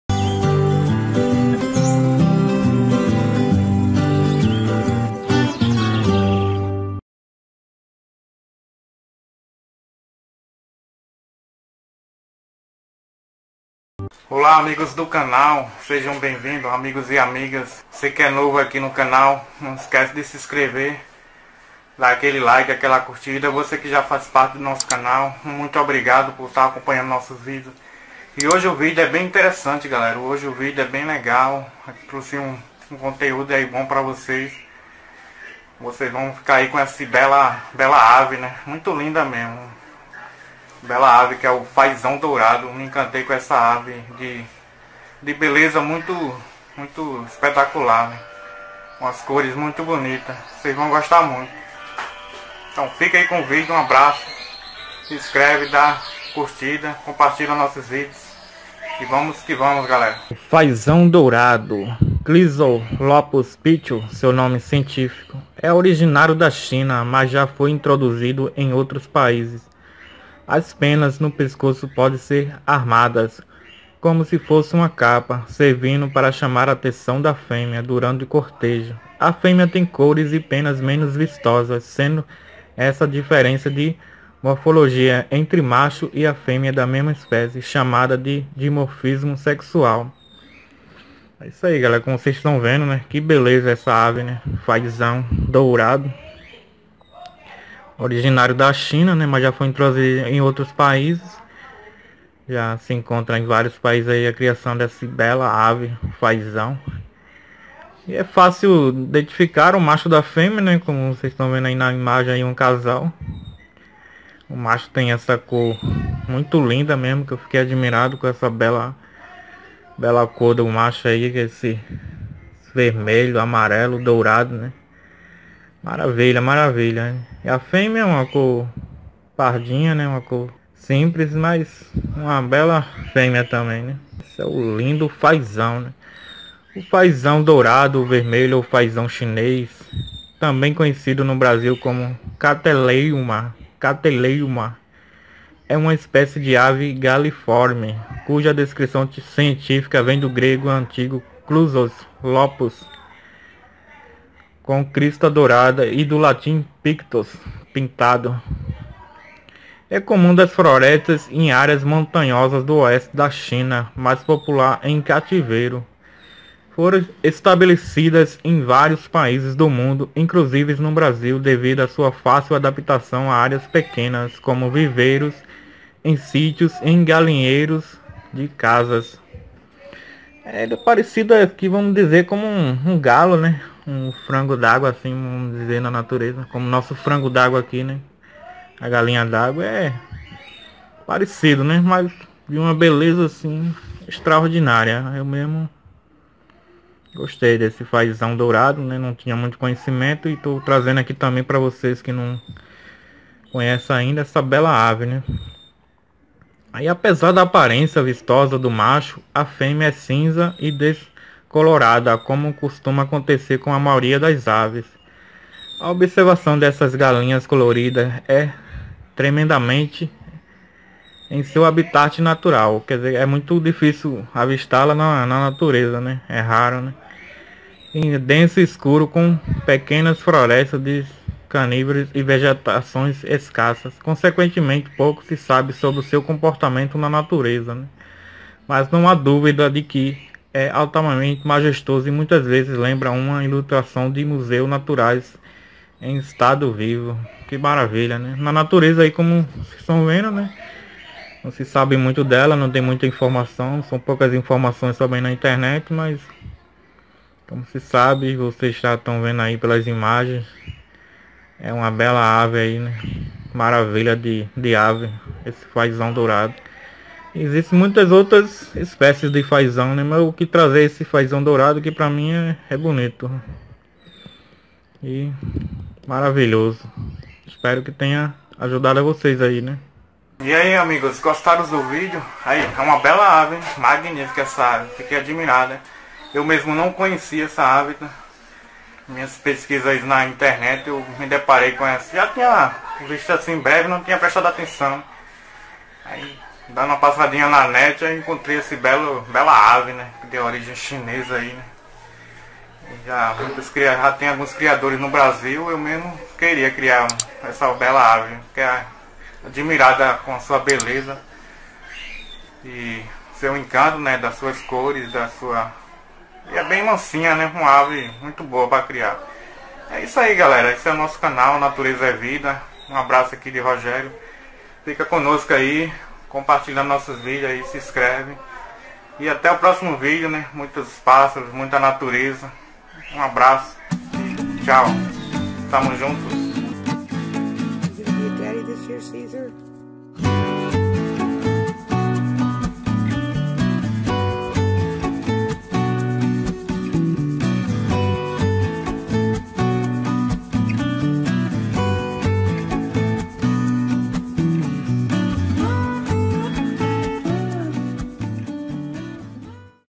CONHEÇA O FAISÃO DOURADO DA CHINA meet the golden pheasant
nesse vídeo falo sobre essa maravilhosa ave o faisão dourado de um beleza exuberante saiba tudo sobre essa ave assista ate o final tmj galera, Pernambuco Brasil, tmj abraços

O faisão dourado (Chrysolophus pictus)é uma espécie de ave  galiforme da família Phasianidae  originária da  China, Birmânia  e algumas outras partes da  Asia.
"Faisão" é oriundo do grego phasianós, pelo latim  phasianu e pelo  provençal  antigo faisan. Que significa Ave colorida de penas longas.
Todas as espécies de faisão apresentam forte dimorfismo sexual,  sendo o macho maior e mais colorido que a fêmea. Os machos têm também longas penas posteriores, que se assemelham a uma cauda. As fêmeas incubam os ovos e tratam das crias sozinhas.

AJUDE A MANTER O PROJETO DESSE CANAL CURTA NOSSOS VIDEOS E COMPARTILHA PARA NOS AJUDAR- TMJ AMIGOS UM GRANDE ABRAÇO.